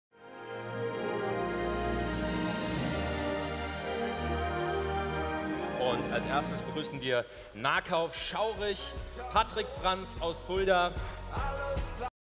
Der REWE Award 2016 geht an Nahkauf Fulda!
Als erster Nahkauf-Markt haben wir den REWE Award gewonnen! Wir sind überglücklich und sagen vielen Dank an alle Mitwirkenden.

Auf viele weiter tolle Aktionen & ein Herz für Kinder!